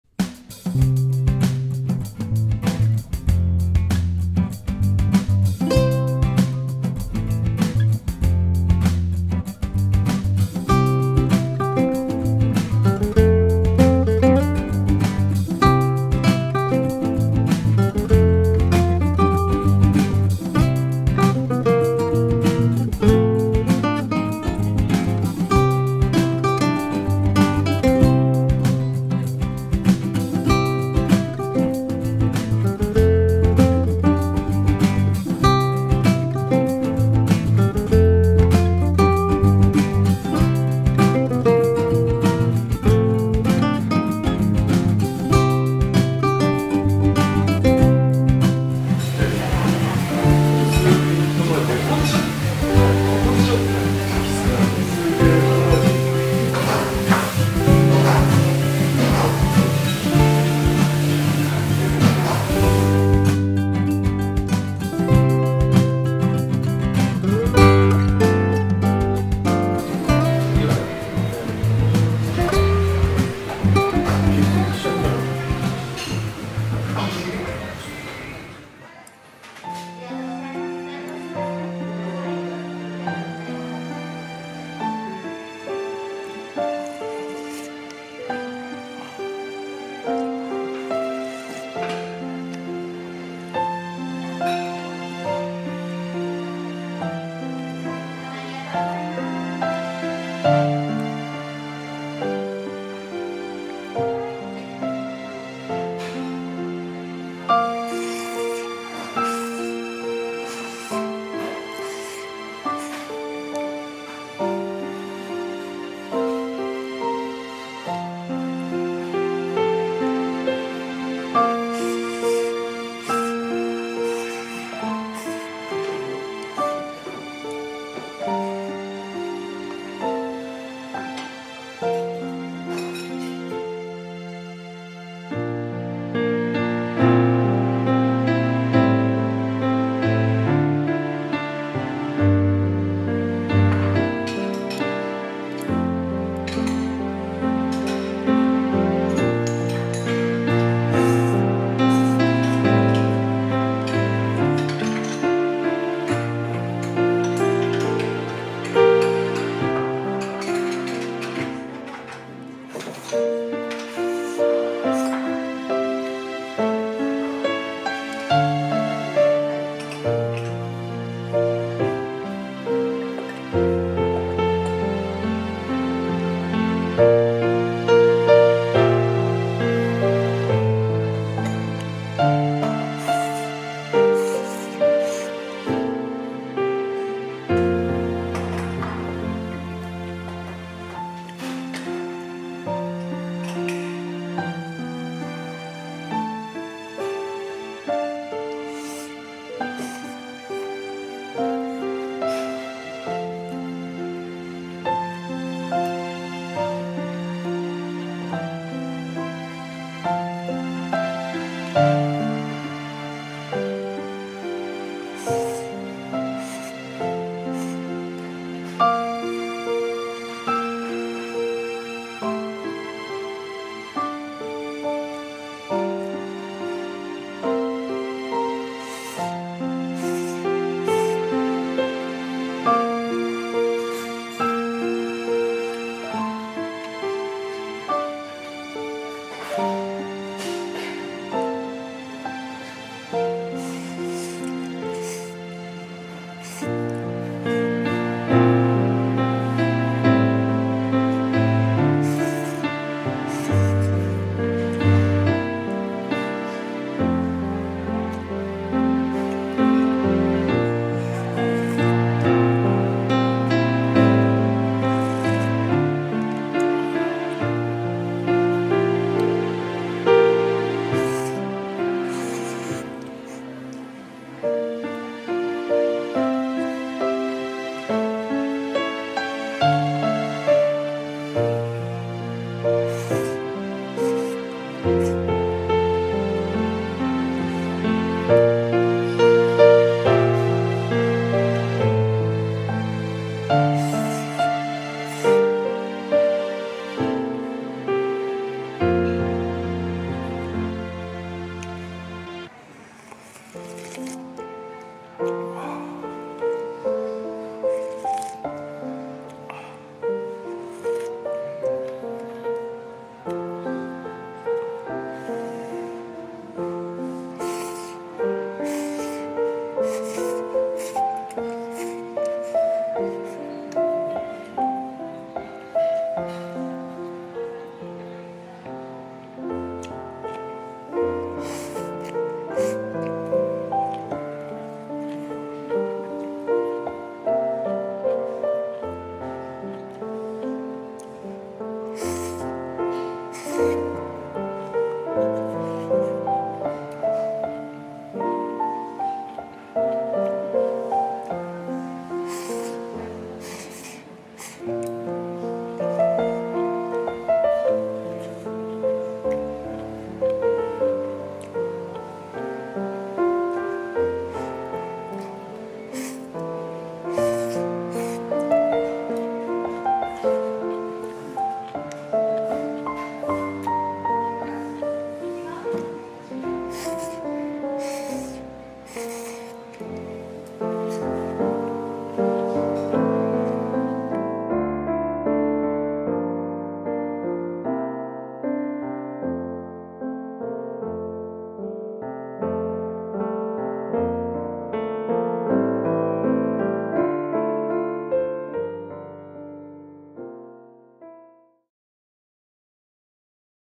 手打ちラーメン福一② 移転しましたでも味はそのままです ＃880
手打ちラーメン福一が、飯山満から馬込沢に3月26日に移転しました
味も変わっていないようで良かったです。